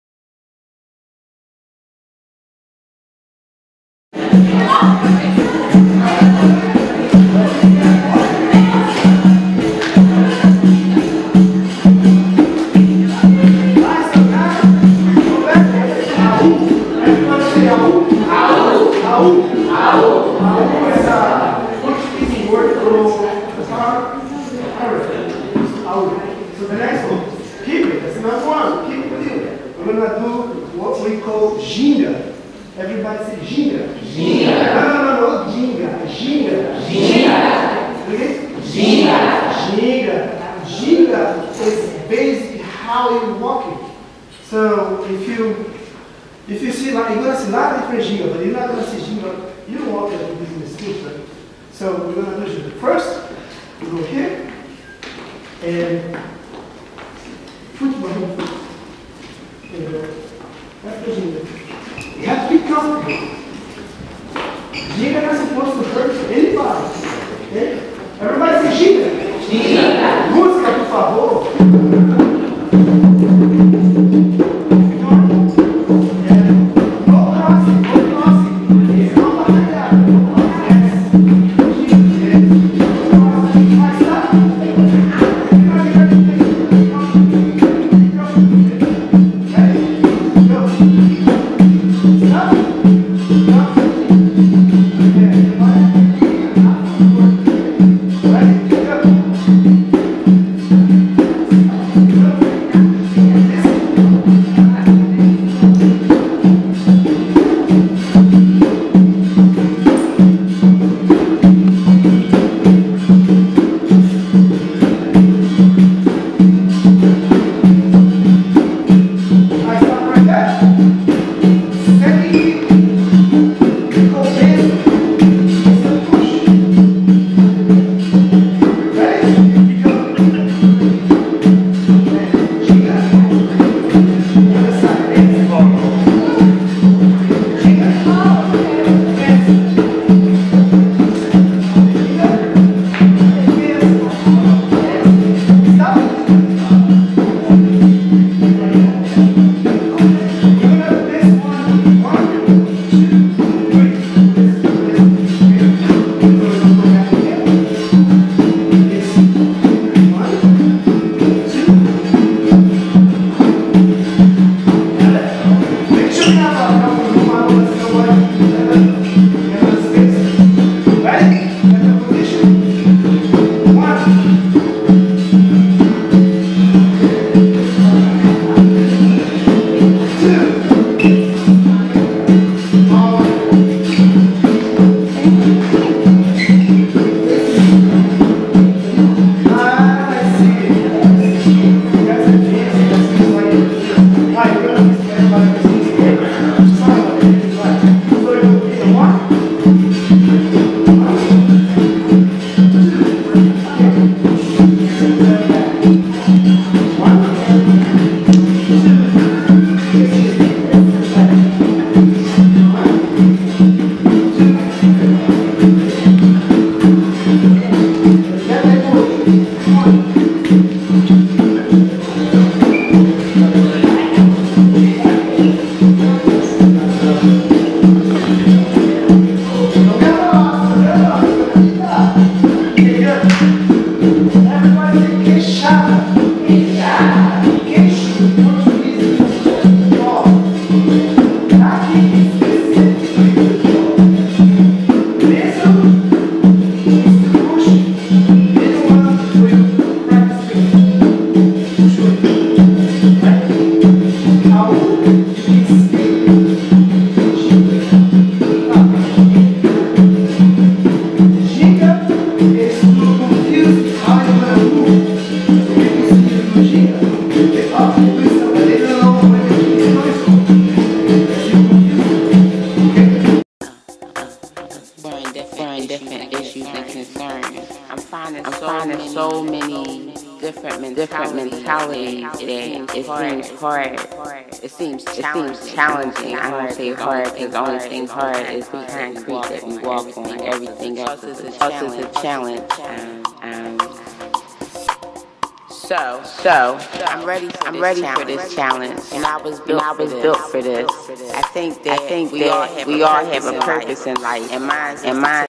Project Capoeira Inc @ Kimmel Center
In honor of black history month the Kimmel Center held a free Capoeira training for youth. We are so fortunate in Philadelphia Pa. to have the Kimmel, they give back so much!!